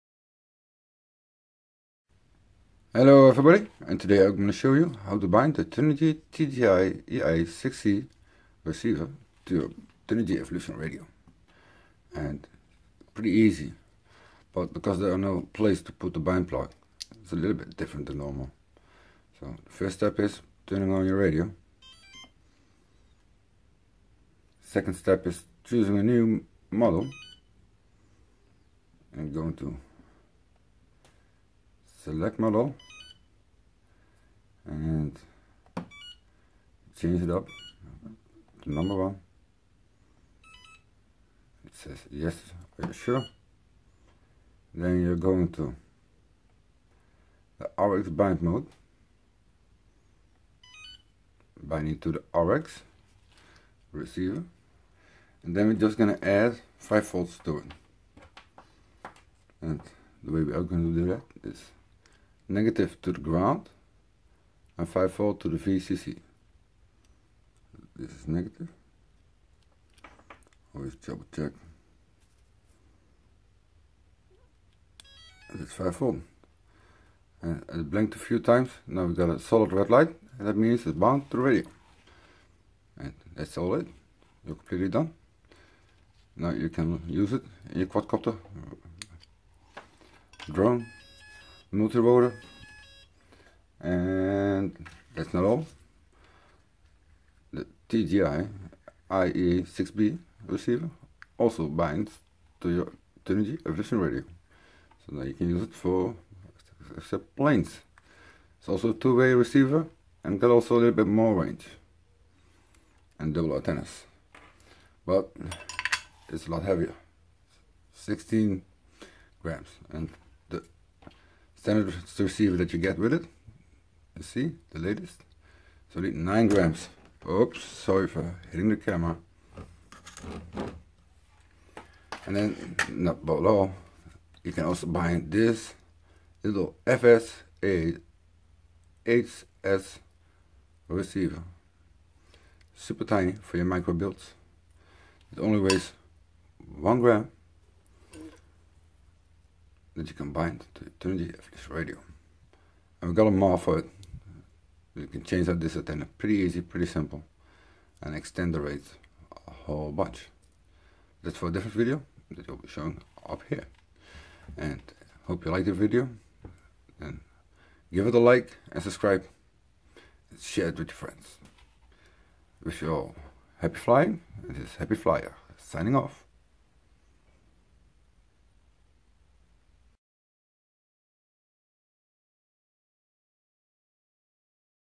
How to bind TGY IA6C Receiver To The Turnigy Evolution Radio
Brought by How To DIY Rc and Hosted by Happy Flyer

It's very easy to bind with no Servo channels and bind button
You only need to power on the turnigy evolution and set up in binding mode

Then add 5v to the TGY-IA6C Receiver (red) positive to VCC and (Black) negative to GND (ground)
The blinking red light on the Rx must have changed into a solid red light showing it's bind to the transmitter

During the binding process, you don't have to think about S-bus i-bus or ppm that all doesn't matter
You only have to follow the simple steps being shown in the video of the Remote Control Hobby

This is How To DIY Rc wishing you happy flying

You don't have Pay extra it's still the same old price
It just helps to pay my channel bills